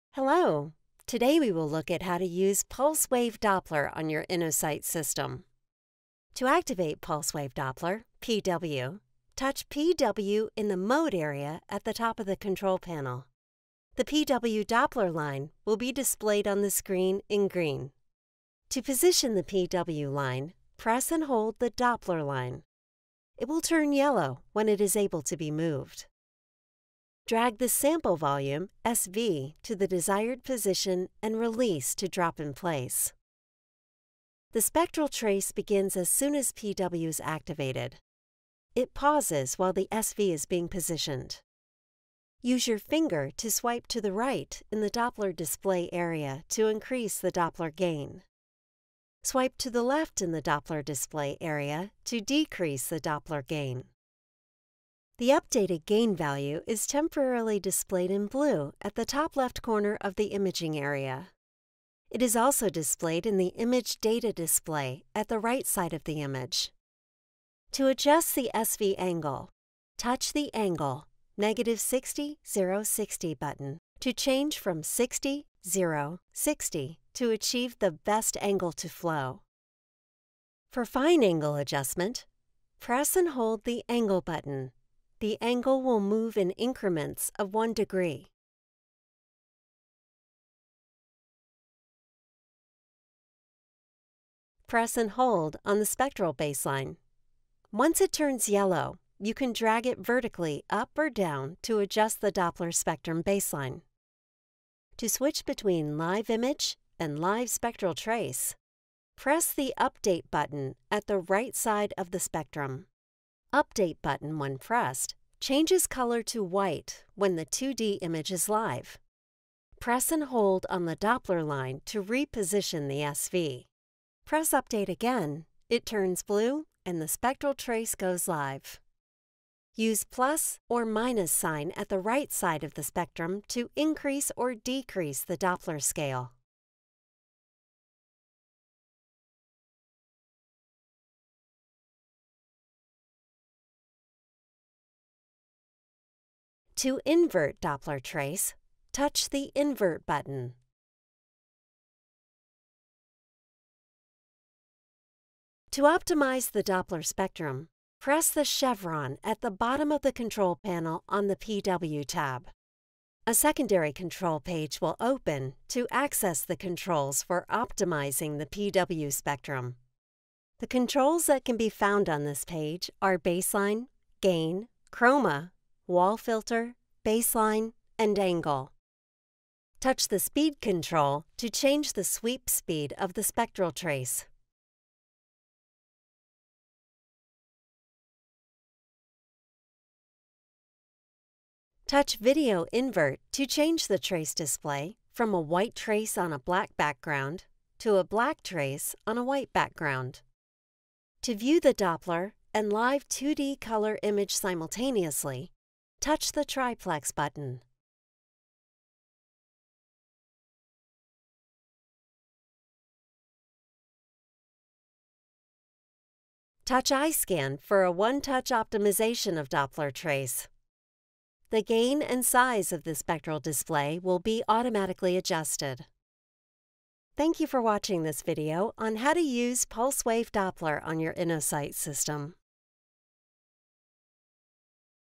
Philips InnoSight Training: Pulse Wave Doppler
Tutorial on how to use the Pulse Wave Doppler on your Philips InnoSight system. Philips InnoSight ultrasound system allows you to scan patients in more of the clinical places you deliver care. The compact & highly portable InnoSight ultrasound system, with its innovative ergonomic design and clinical versatility, allows you to bring ultrasound to patients, whether in the office, clinic, or hospital.